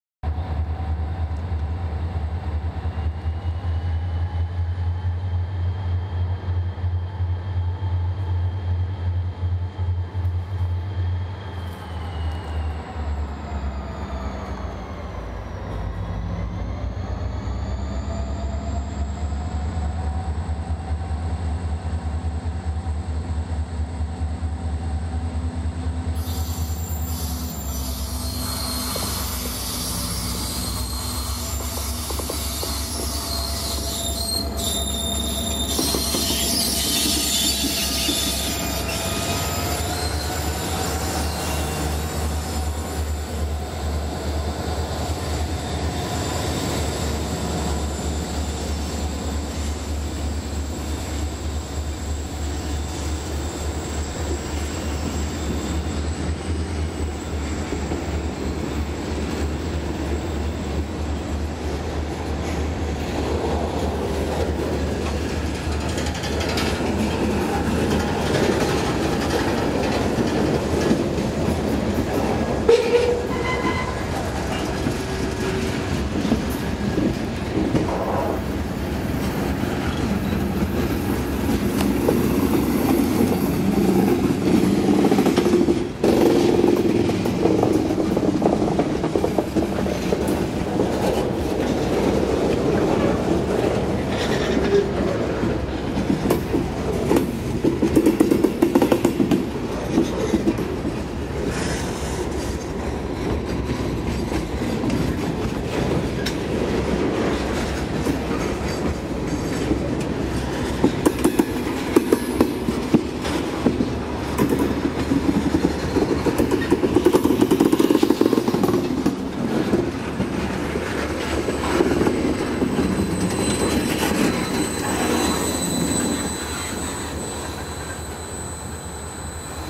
NS H76 rumbles through Martin's Creek
We are in Martin's Creek, where we see Norfolk Southern Train H76, the local out of Allentown, as it heads up the B&P towards Portland and Slateford.  Today's consist is SD40-2's 3347 and 3429 with a 16 car mixed freight.  Once the train reaches Portland, it backs down the wye and into Slateford, where it will interchange with the DL.  We will see the DL later on.  Enjoy the sound of the turbocharged rev of the 645's.  If you have any questions about the video, feel free to message me.  Comments are welcome.  The video was taken on May 4th, 2013.